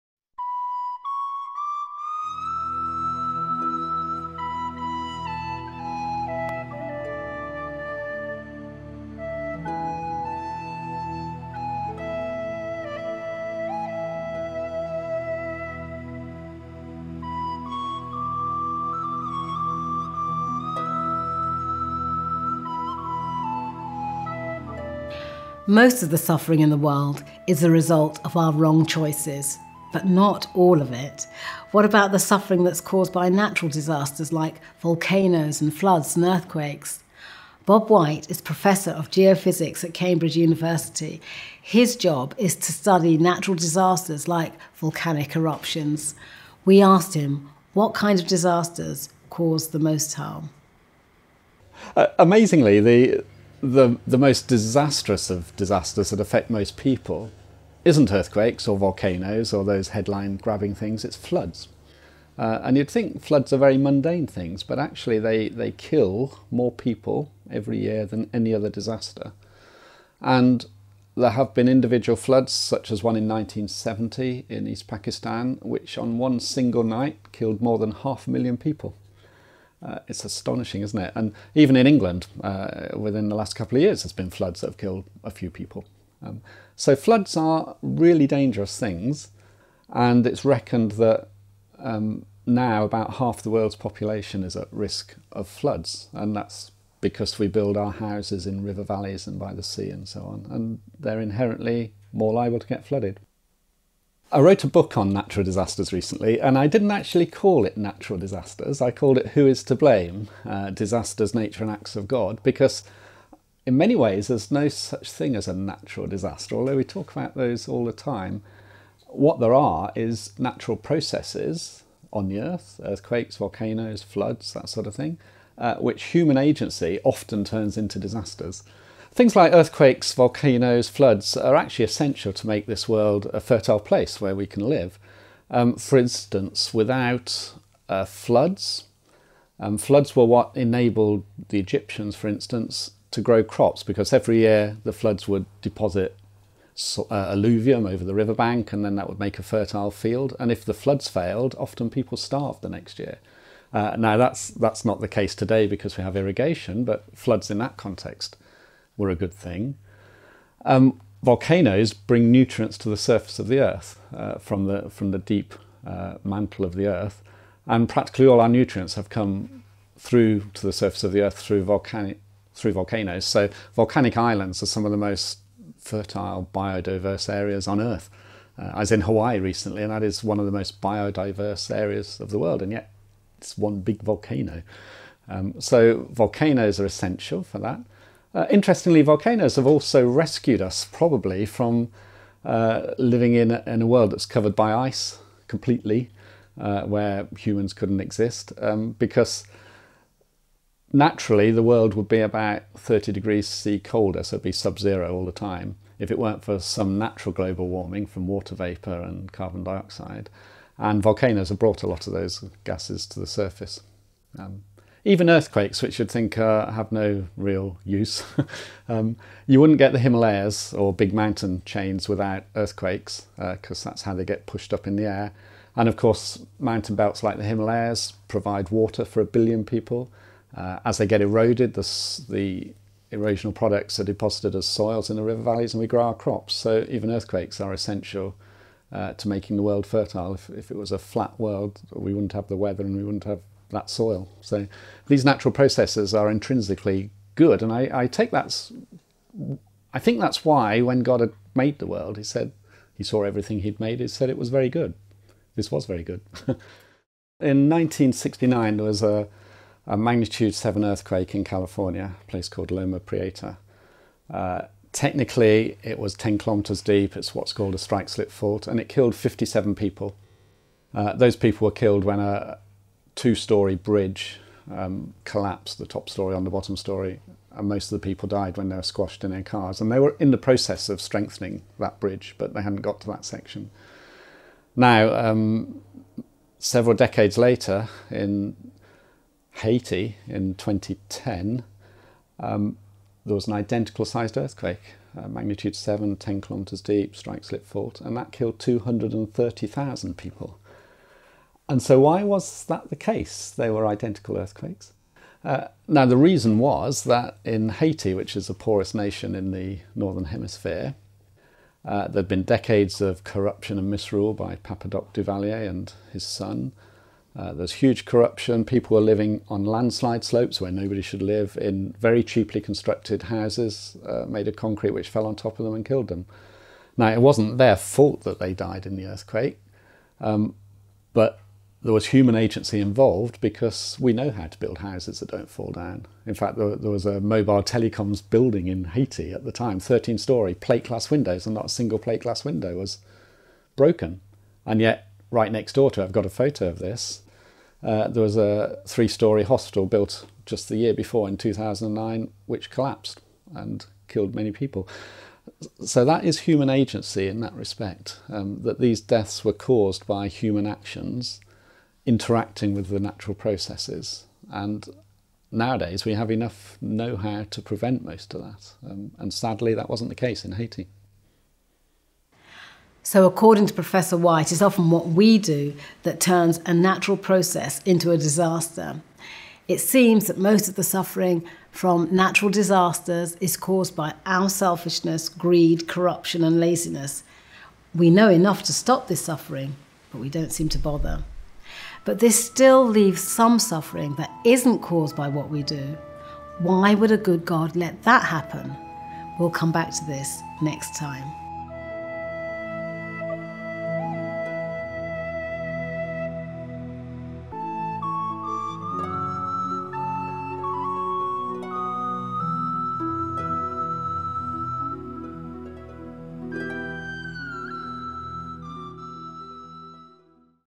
Natural Disasters - ch11 of 'The Biggest Question: why does God allow suffering?'
Why does God allow the suffering caused by natural disasters like floods, earthquakes, volcanoes and tsunamis? Bob White is professor of Geophysics at Cambridge University, and author of ‘Who is to Blame? Disasters, nature and acts of God.’ He studies natural disasters such as volcanic eruptions. In this video, Bob White argues that most of the suffering in natural disasters is a result of our own selfishness, greed, corruption or laziness. Part 11 of ‘The Biggest Question: why does God allow suffering?’ introduced by BBC TV Songs of Praise presenter Diane Louise Jordan.